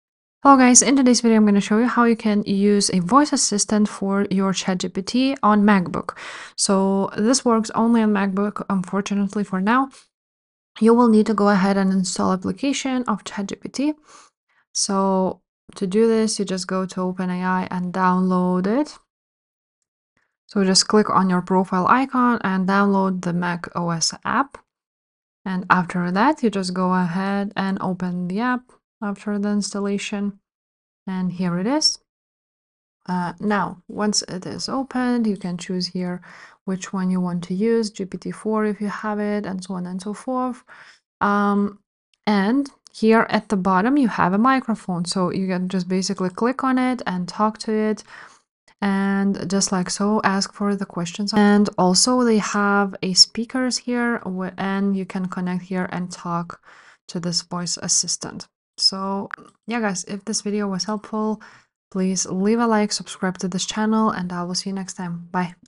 How To Use Voice Control For ChatGPT On Mac
In this video I show you How To Use Voice Control For ChatGPT On Mac!